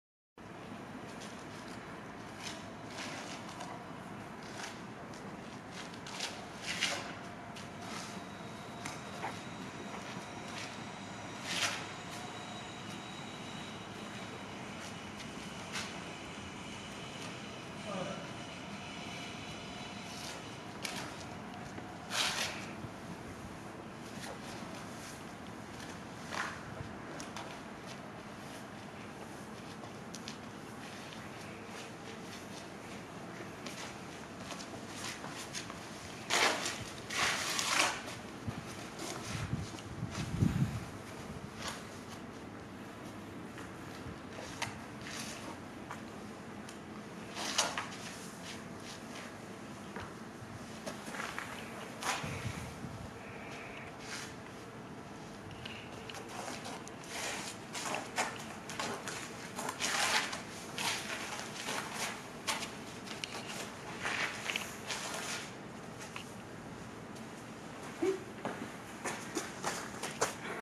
02 Carlswerk Corona
Improvisieren mit Hannah aufm Carlswerk Gelände am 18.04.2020. Die Zweite.